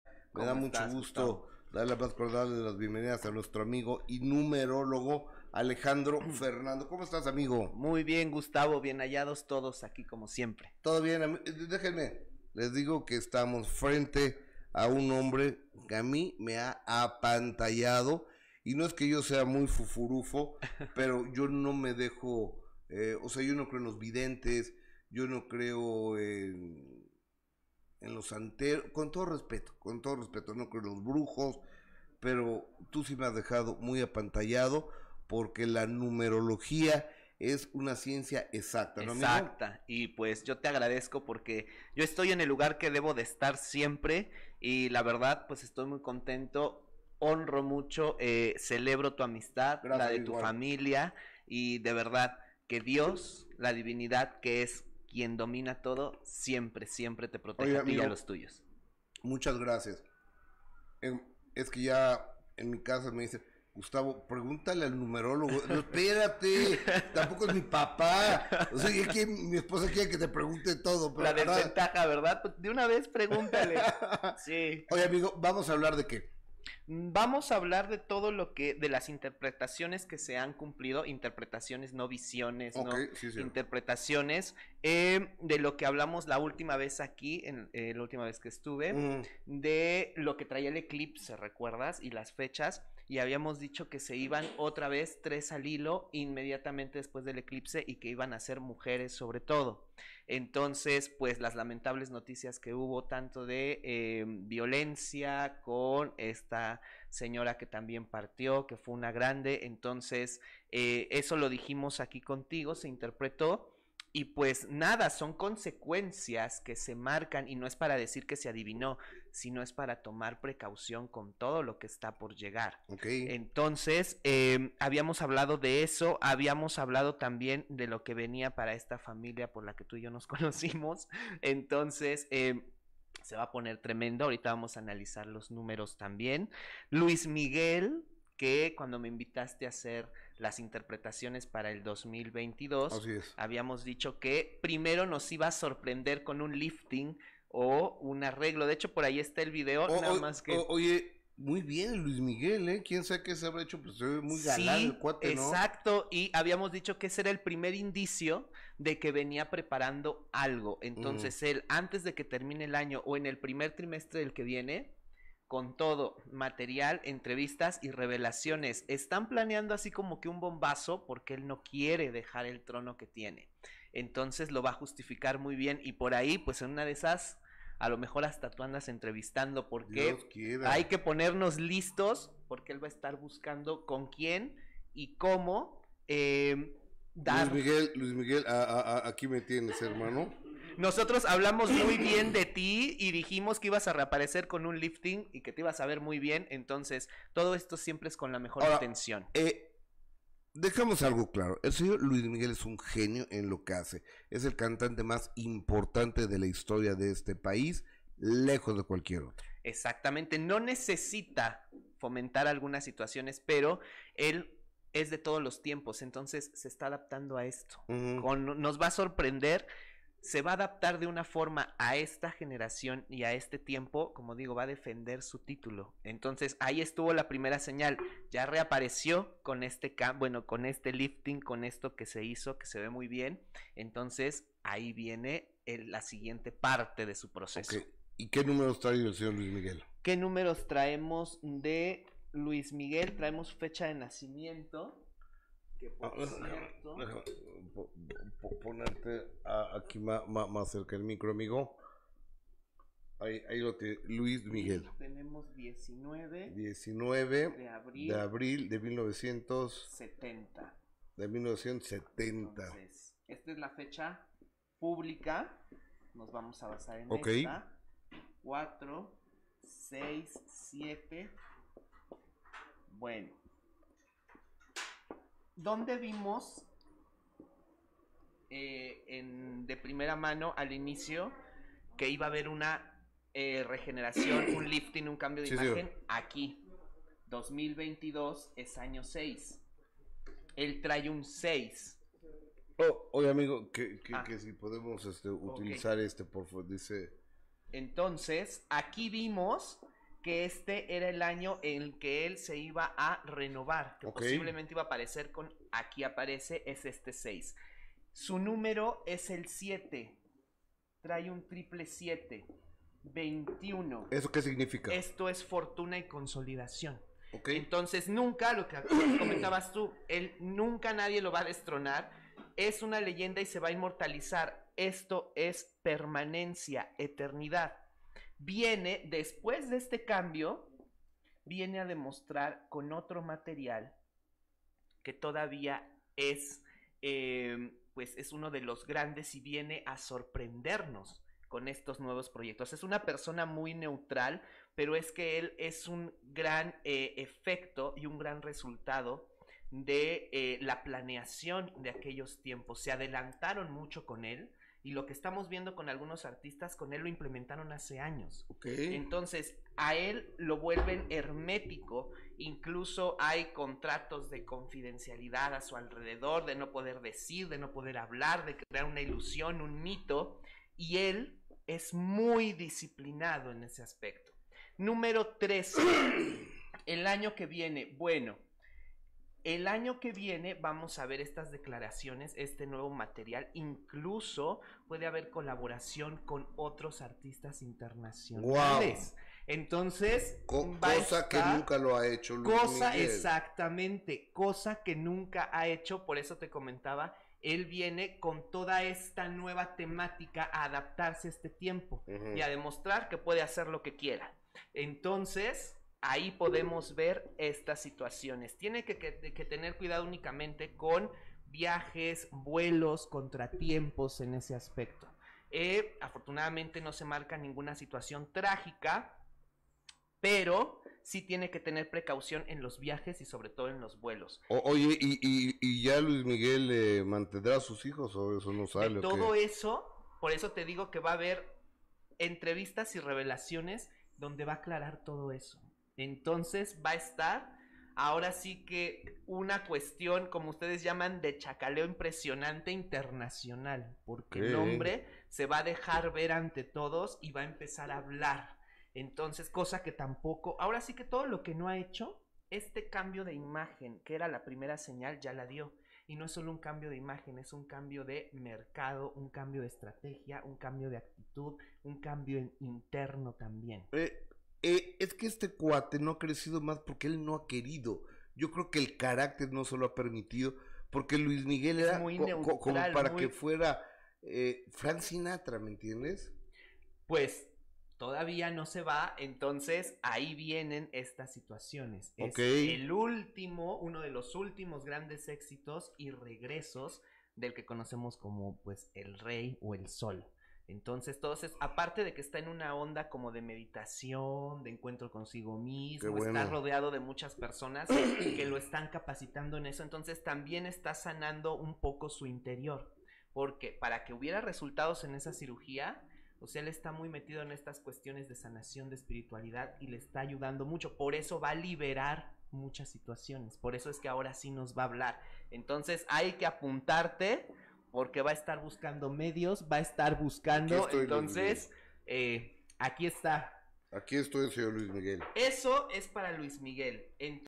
¡Luis Miguel contará su HISTORIA con Aracely Arámbula! ¡Esto dice el numerólogo Alejandro Fernando!
Númerologo hace grandes revelaciones sobre LuisMiguel, quien podría sorprender con nuevo material musical, además de grandes colaboraciones internacionales…. ¿Contará su historia con AracelyArámbula?